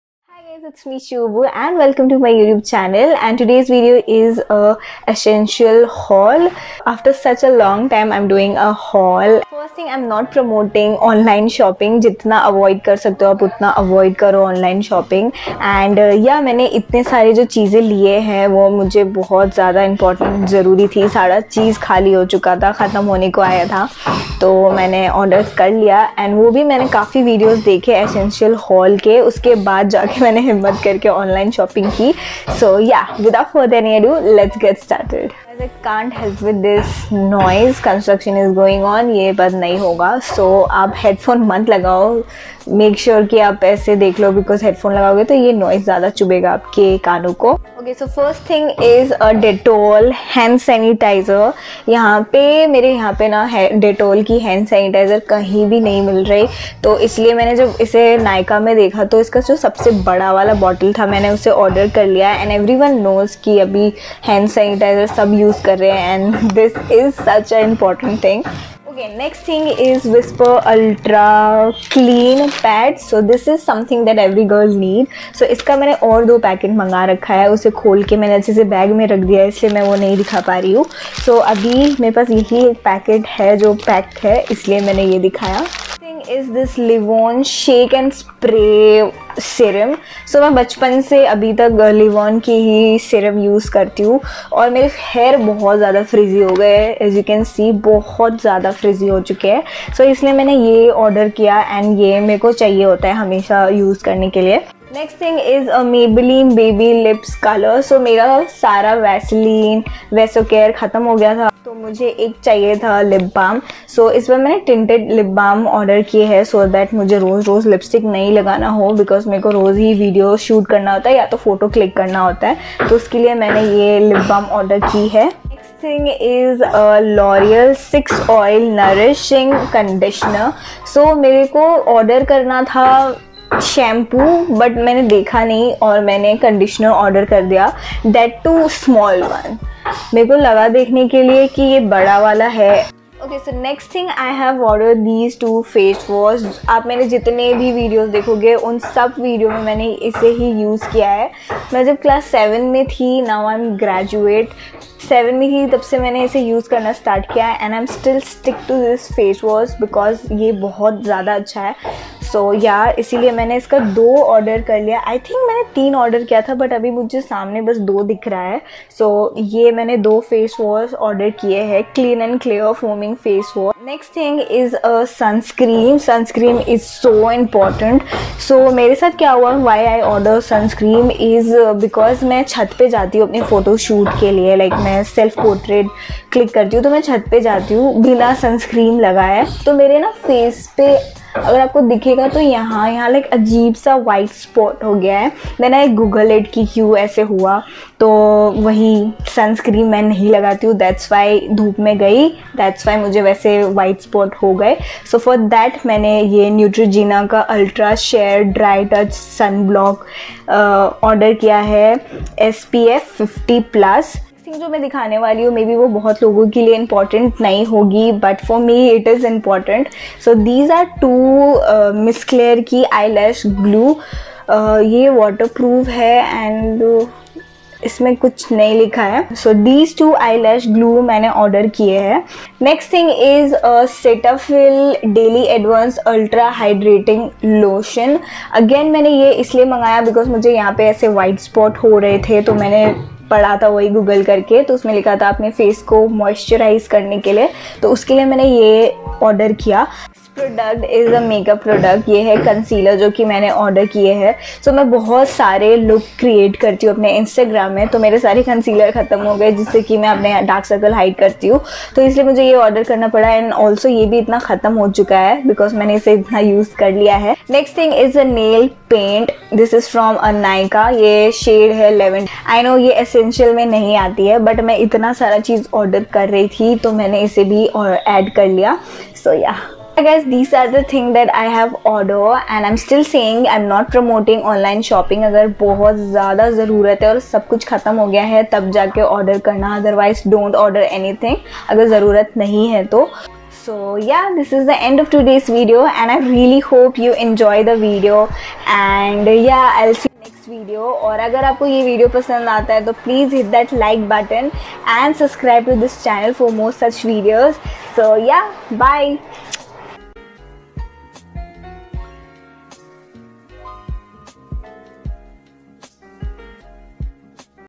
Nykaa ESSENTIALS Haul | Lockdown Edition | hey its shobu
Lockdown essentials Haul /things I cannot live without

!! NEW VIDEO TWICE A WEEK !!

Product links!!

 Maybelline New York Baby Lips Color Balm SPF 20 - Berry Crush

 Miss Claire Eyelash Glue Adhesive Dark Tone on Nykaa

Whisper Ultra Clean Sanitary Pads - XL+ (44 Pc Pack) Offer on Nykaa

Maybelline New York Instant Age Rewind Eraser Dark Circles Treatment Concealer - Caramel on Nykaa

Music
Audio labary